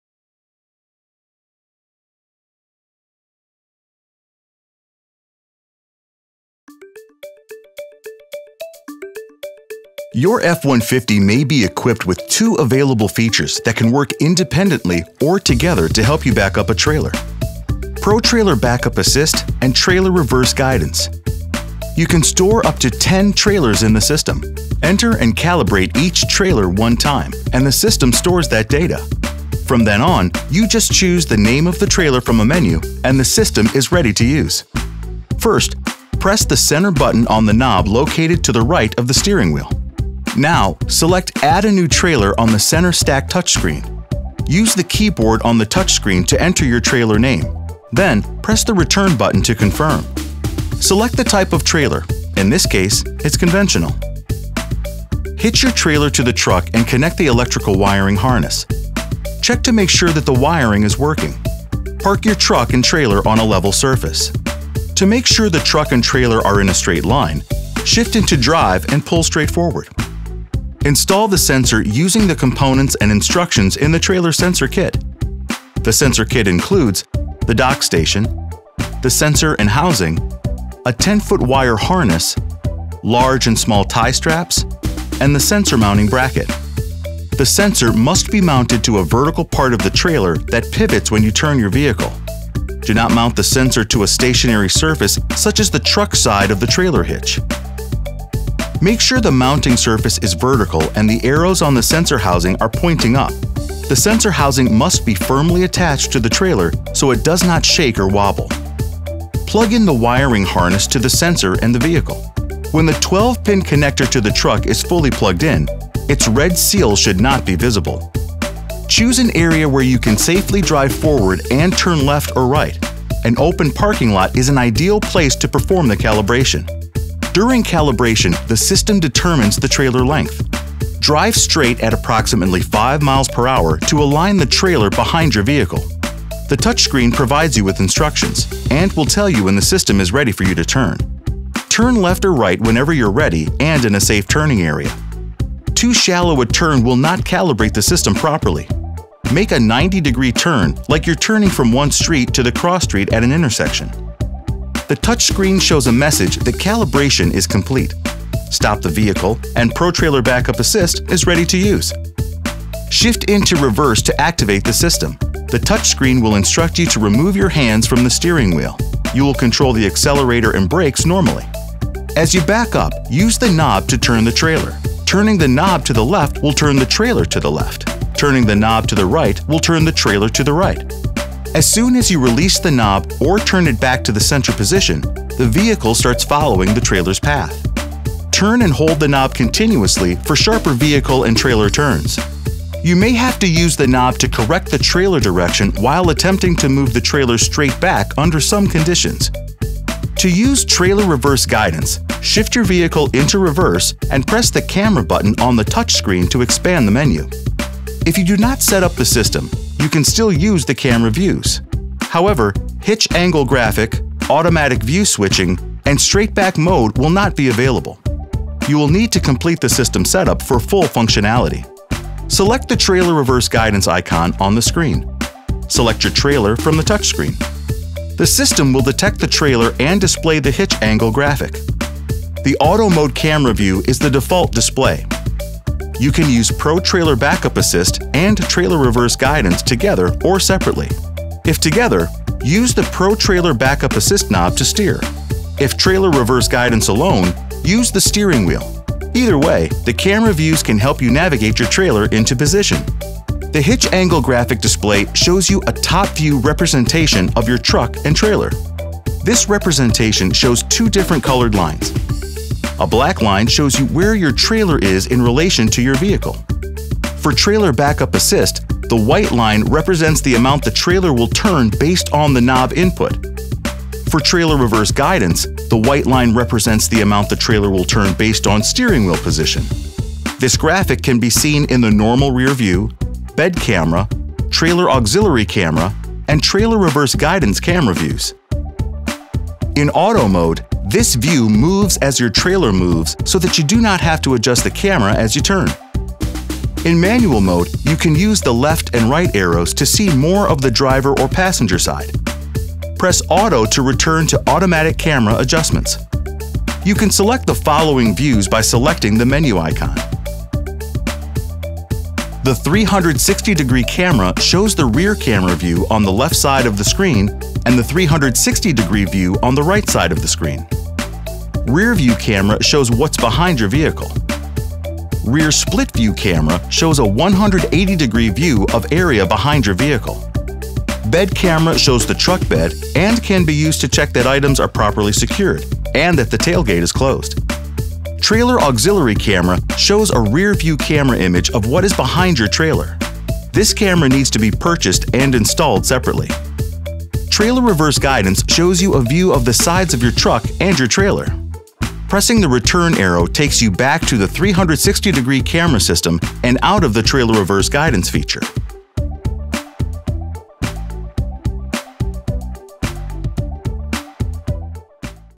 Pro Trailer Backup Assist™ With Trailer Reverse Guidance: Setup & Use
Your F-150 may be equipped with two available features that can work independently or together to help you back up a trailer.